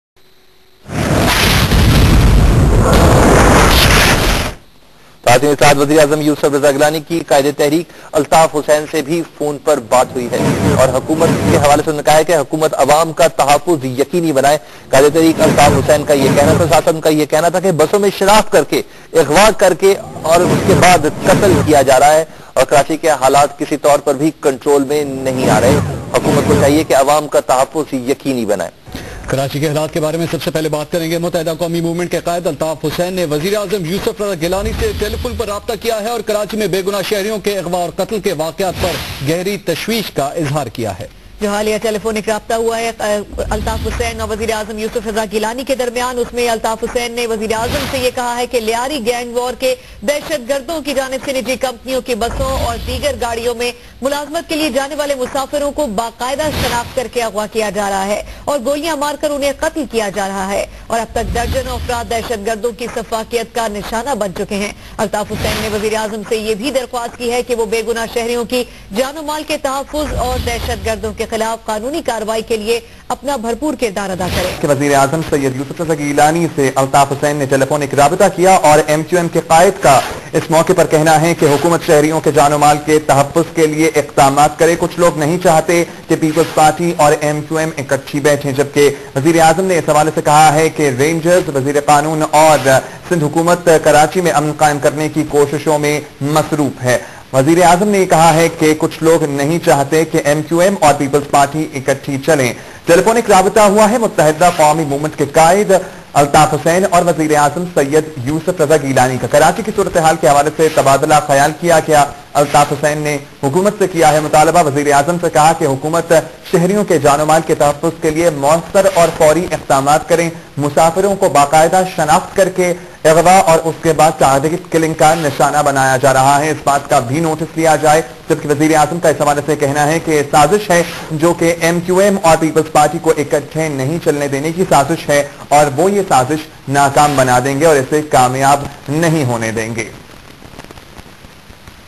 Altaf Hussain asks PM Gillani to protect hapless citizens in Karachi
Founder and Leader of Muttahida Quami Movement (MQM) Mr Altaf Hussain telephoned Prime Minister Gillani and conveyed his deep concerns over the abduction and killing of innocent people in Karachi. Mr Hussain informed Prime Minister Gillani that the terrorists of the gang war were kidnapping and killing passengers from buses of private companies and other vehicles that were going to their offices after ascertaining their identities. He told Prime Minister Gillani that dozens of people have fallen victim to butchery of the terrorists. He requested the Prime Minister to play his role to protect the life and property of innocent citizens and action against the terrorist.

Prime Minister Gillani strongly condemned the killing of innocent people in Karachi and said that the killing of innocent citizens was a tyranny. He said that he was grieved at the situation in Karachi. He said that certain people do not want the PPP and the MQM to work together, but the government would not let their conspiracies to succeed. He assured Mr Hussain that the government will discharge its full responsibilities for protecting the life and property of innocent people in Karachi. He said that instructions have already been issued to Chief Minister Sindh Syed Qaim Ali Shah and officials of Rangers and other law-enforcing agencies.